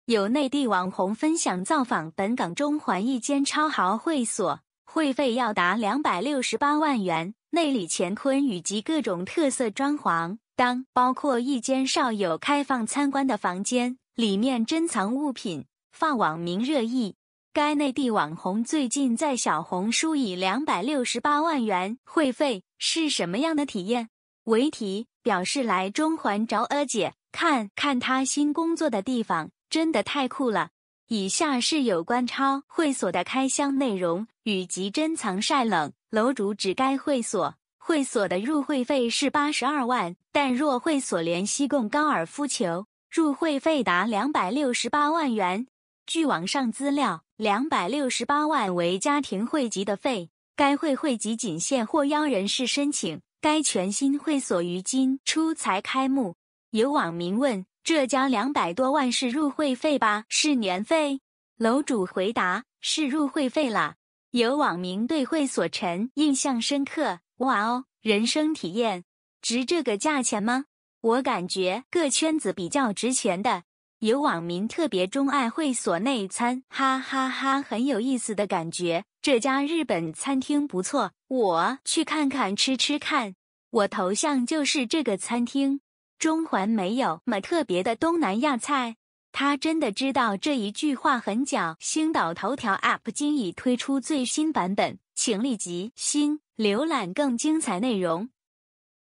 268萬入會費！中環超豪會所內部曝光 呢樣收藏全港絶無僅有｜Juicy叮
268萬入會費！中環超豪會所內部曝光 呢樣收藏全港絶無僅有｜Juicy叮